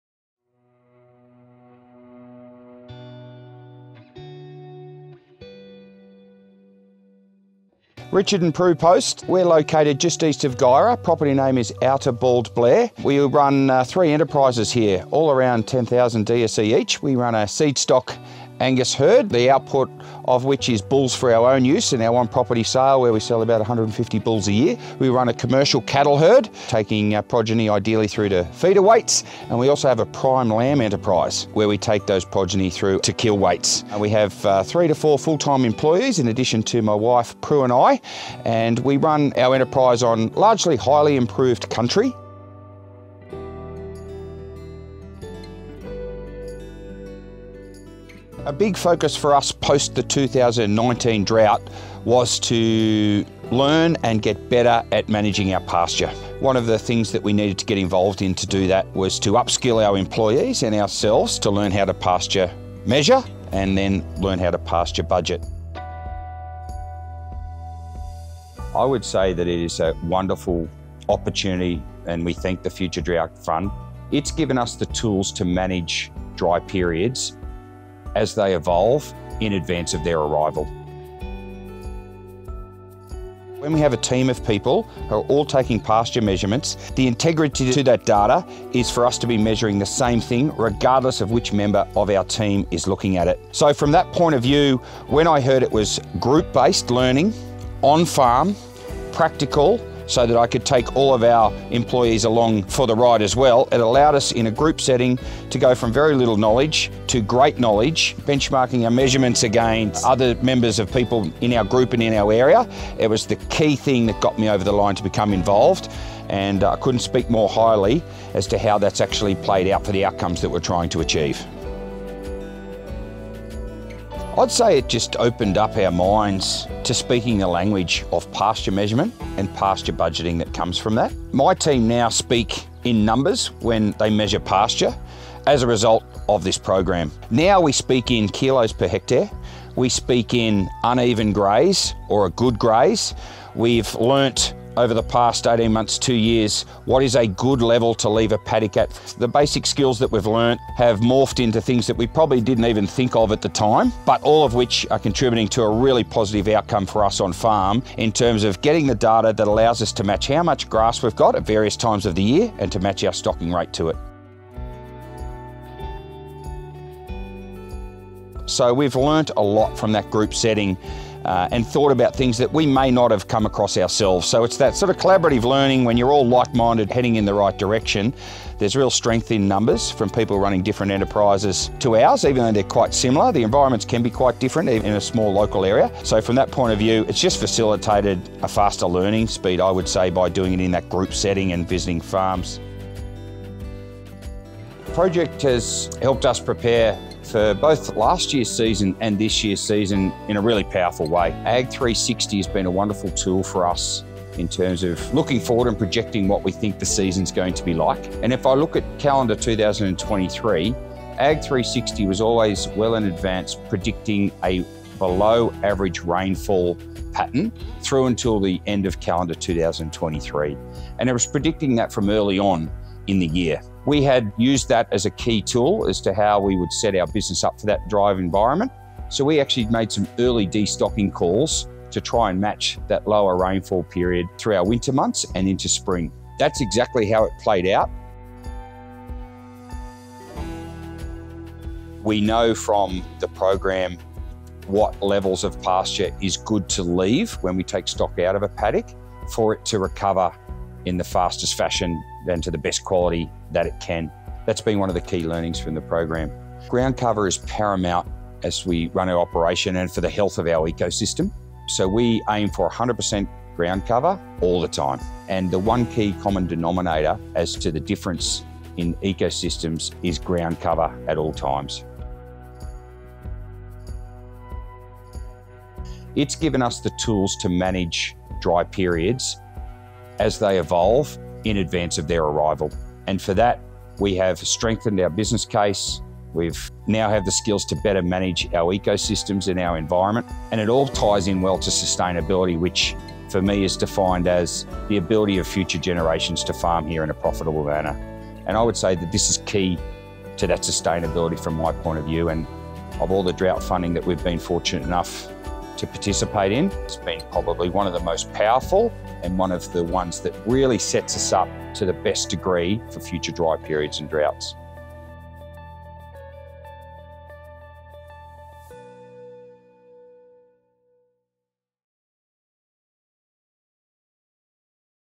Coaching boosts sustainable land management skills
In this video, Richard and Prue Post discuss their farming operations at Outer Bald Blair, located east of Guyra NSW. By participating in a coaching program run by the University of New England and supported by the Future Drought Fund, they and their team learned how to measure and budget pasture, which has been crucial for managing their land sustainably. Their focus has been on improving pasture management, especially after the 2019 drought.

The coaching program emphasised practical, group-based learning, which allowed the Posts to benchmark their progress against others and rapidly improve their skills. This has helped them make informed decisions, like early destocking in anticipation of lower rainfall, guided by tools such as Ag360 (ag360.com.au).

This project was funded by the Australian Government's Future Drought Fund – Drought Resilient Soils and Landscapes (DRSL) Grants Program and hosted by the University of New England, with on-ground partners GLENRAC Inc, Southern New England Landcare Ltd, and Northern Tablelands Local Land Services.

You can find all the resources developed during this project – including manuals to help you run a similar process in your area, case studies, pasture assessment and water quality information